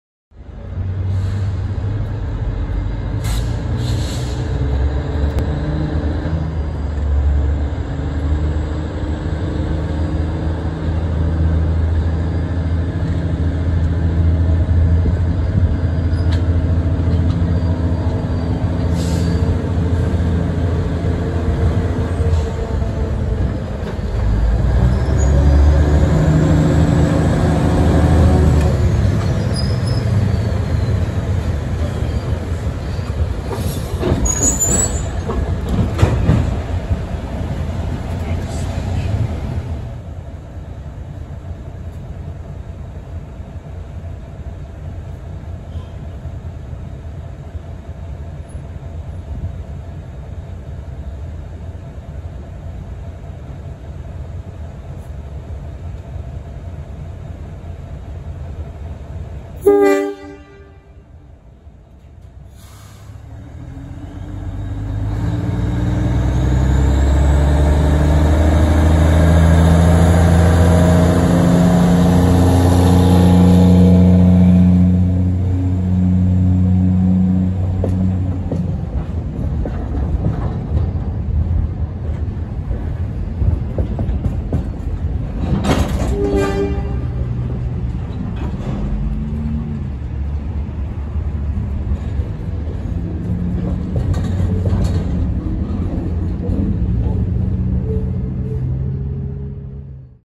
MTA NYC Subway Work Train Leaving Rail Bending Shed at 36th-38th St Yard
Here are two videos taken in March 2022, featuring the 36th-38th St Yard in Sunset Park, Brooklyn. A work train can be seen leaving the rail bending shed facility at this massive yard, which is generally used to store work trains and load work trains for transporting materials to work sites, like rails.